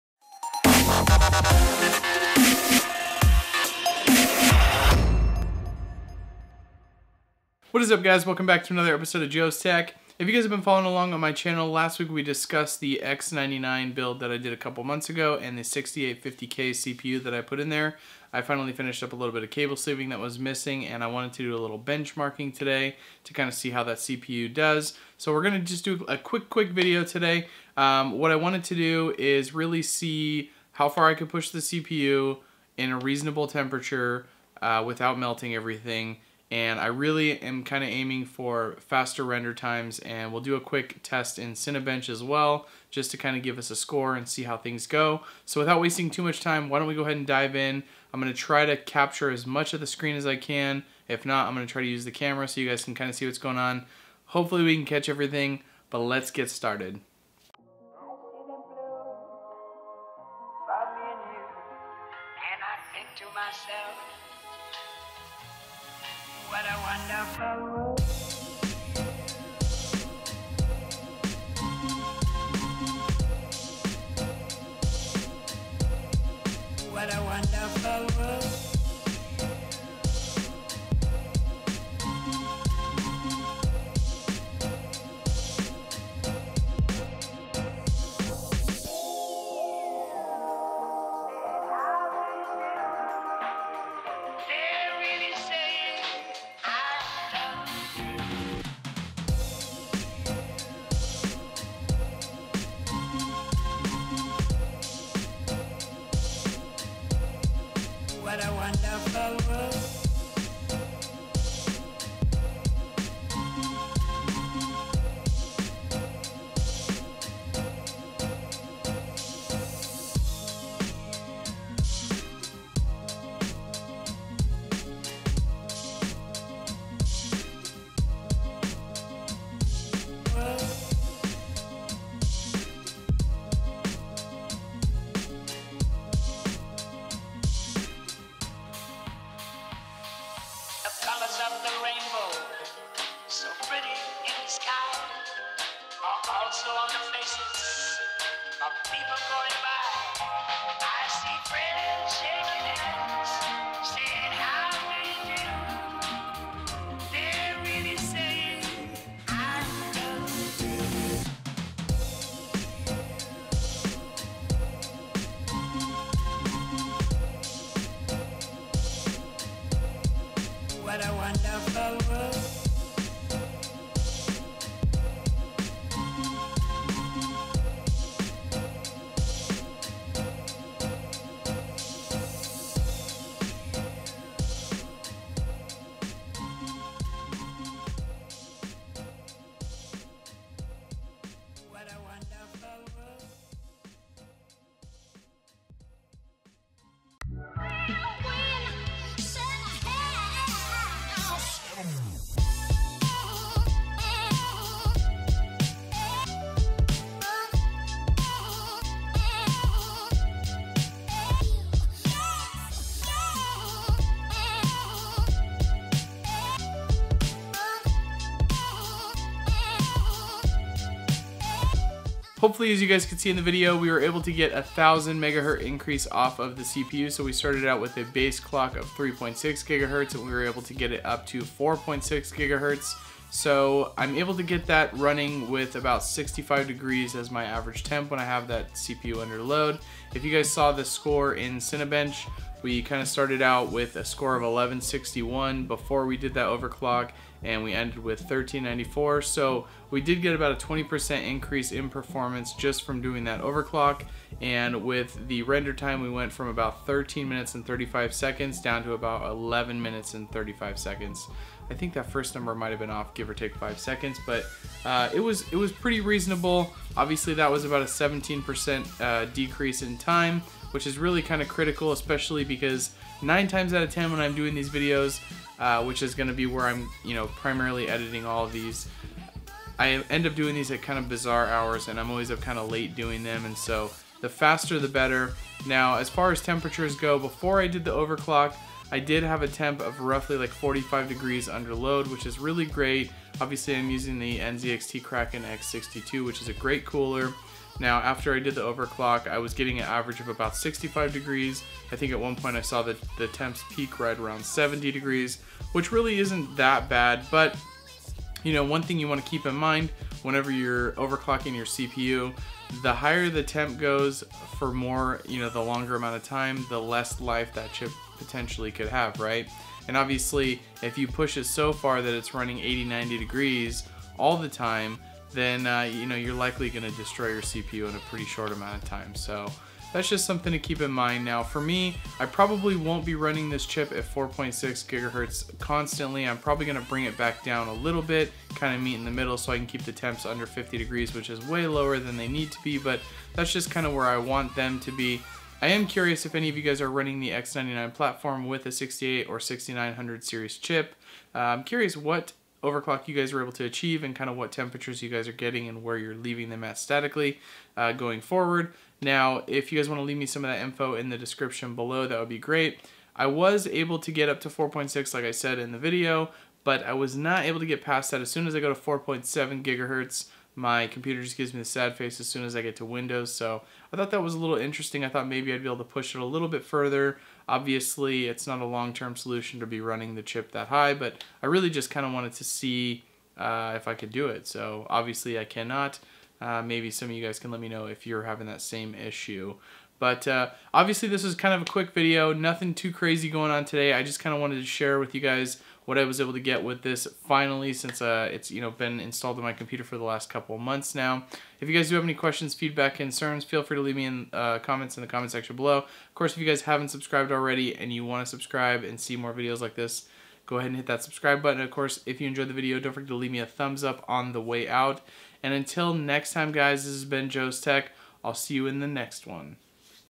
Overclocking my 6850K - Render testing and benchmarking
What is up guys, today we are looking at my not so new Intel 6850K strapped into my MSI x99 Titanium motherboard.  We will just be doing a quick overclock to see if there is a difference in render times between the 3.6GHz base and wherever we can boost it up to.

Intel i7 6850k
MSI Titanium X99A
Dominator Platinum Ram

Ethics Statement: I am a member of the Amazon Associates Program.